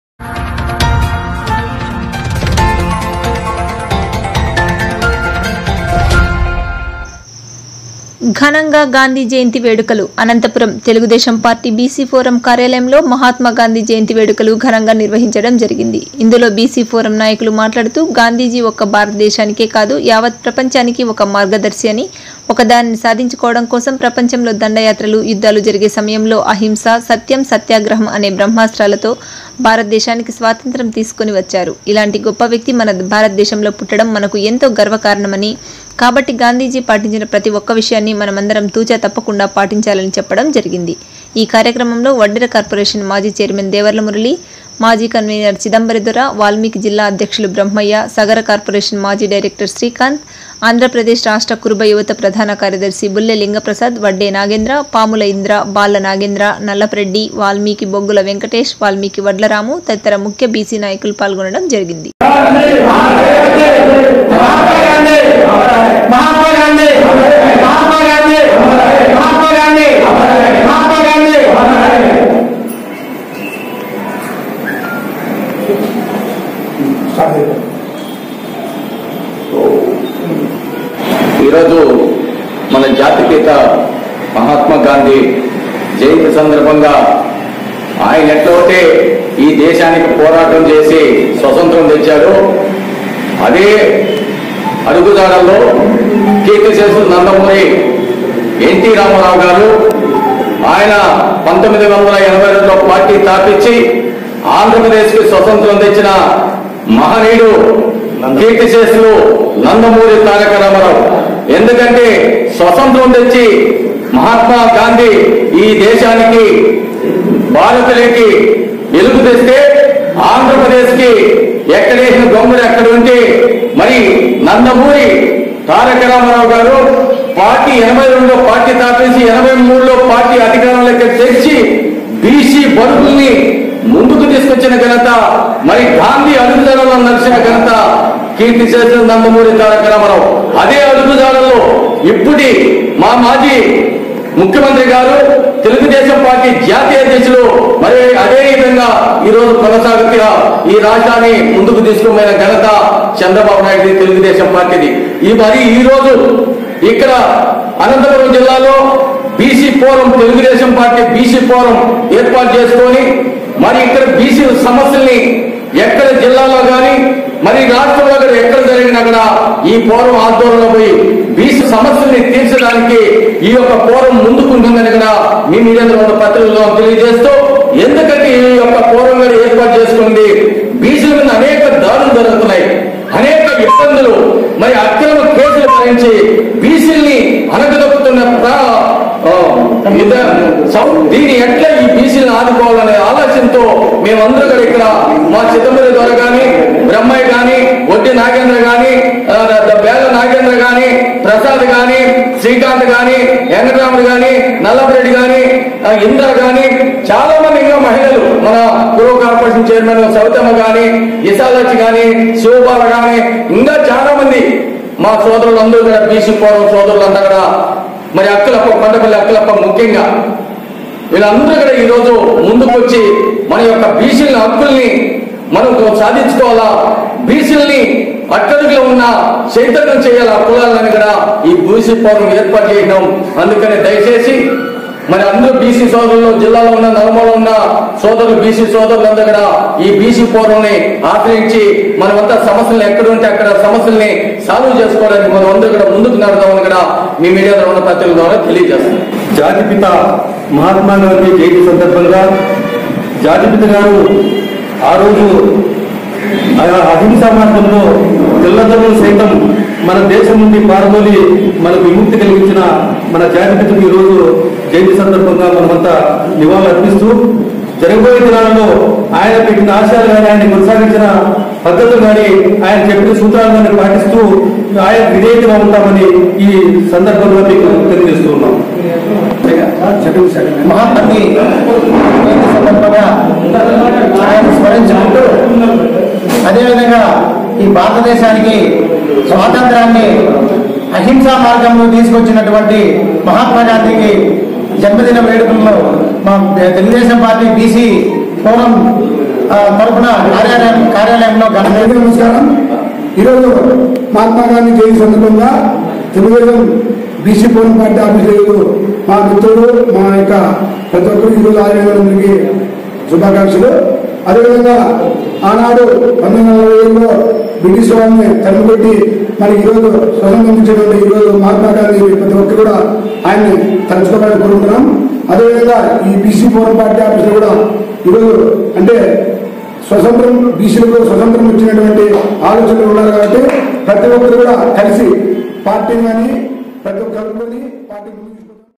ఘనంగా గాంధీ జయంతి వేడుకలు ®RAMOJ TV®
అనంతపురం

తెలుగుదేశం పార్టీ బిసి ఫోరం కార్యాలయం కార్యాలయంలో మహాత్మా  గాంధీ జయంతి వేడుకలు ఘనంగా నిర్వహించడం జరిగింది.ఇందులో బీసీ ఫోరం  నాయకులు మాట్లాడుతూ గాంధీజీ ఒక్క భారతదేశానికే కాదు యావత్ ప్రపంచానికి ఒక మార్గదర్శిని,ఒక దానిని సాధించుకోవడం కోసం ప్రపంచంలో దండయాత్రలు యుద్ధాలు జరిగే సమయంలో అహింస, సత్యం, సత్యాగ్రహం అనే బ్రహ్మాస్త్రాలు తొ భారత దేశానికి స్వాతంత్య్రం తీసుకుని వచ్చారు. ఇలాంటి గొప్ప వ్యక్తి మన భారతదేశంలో పుట్టడం మనకు ఎంతో గర్వకారణమని ,కాబట్టి గాంధీజీ పాటించిన ప్రతి ఒక్క విషయాన్ని మనమందరం తూచా తప్పకుండా పాటించాలని చెప్పడం జరిగింది.
ఈ కార్యక్రమంలో వడ్డెర కార్పొరేషన్ మాజీ చైర్మన్ దేవళ్ల మురళీ,మాజీ కన్వీనర్ చితంబరి దొర,వాల్మీకి జిల్లా అధ్యక్షులు బ్రహ్మయ్య,సగర కార్పొరేషన్ మాజీ డైరెక్టర్ శ్రీకాంత్,ఆంధ్రప్రదేశ్ రాష్ట్ర కురుబ యువత  ప్రధాన కార్యదర్శి బుల్లె లింగ ప్రసాద్,వడ్డే నాగేంద్ర ,పాముల్లా ఇంద్ర,బాళ్ల నాగేంద్ర,నల్లప్ప రెడ్డి ,వాల్మీకి బొగ్గుల వెంకటేష్,వాల్మీకి వడ్ల రాము తదితర ముఖ్య బీసీ నాయకులు పాల్గొనడం జరిగింది.